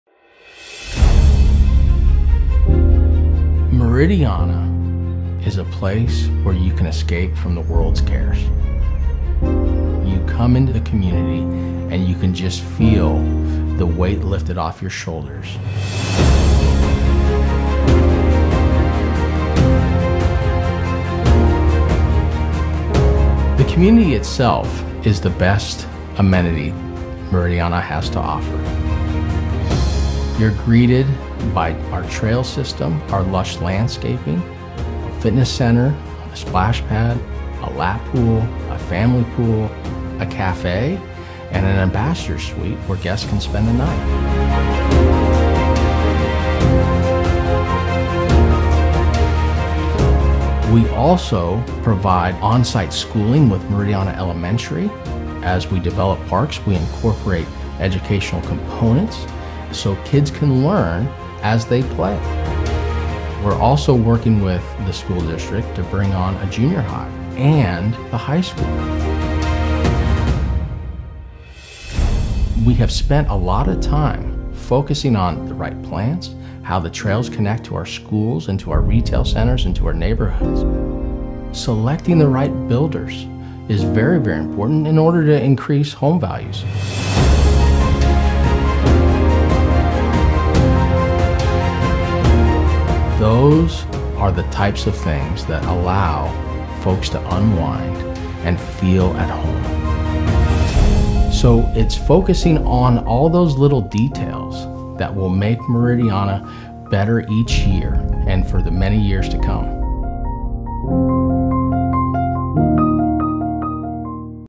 Meridiana - Community Overview
Overview of the Meridiana community in Iowa Colony, Texas.